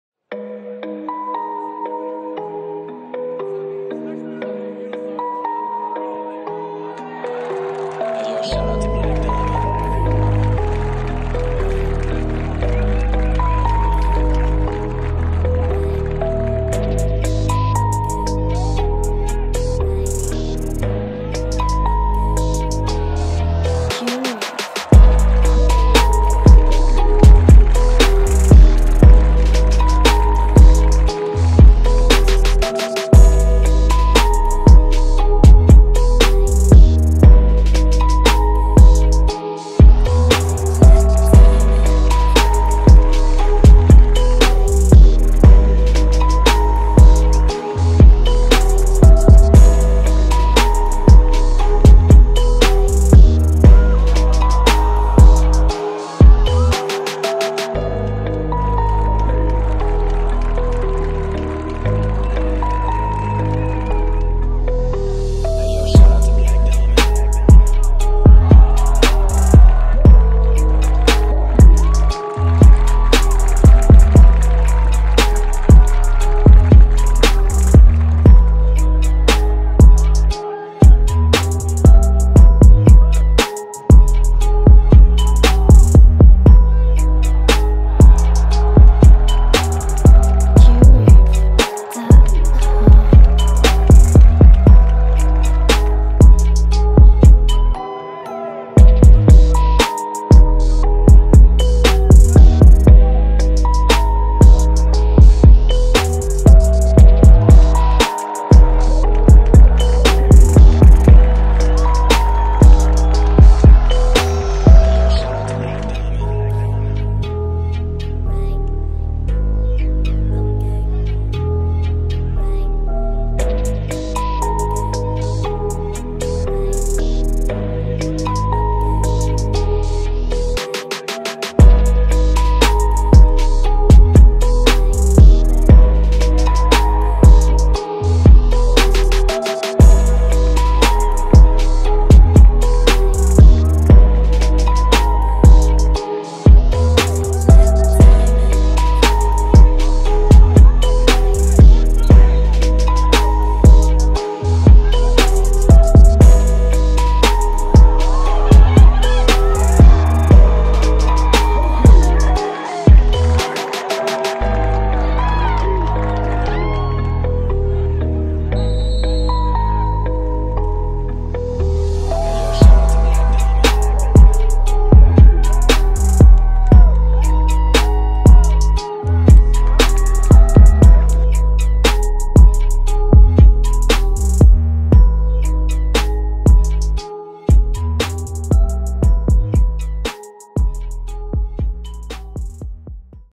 Virginia Tech Men's Soccer Week 6
Virginia Tech Men's Soccer week 6 highlight tape. copyright credit (song prod. by Black Diamond Music)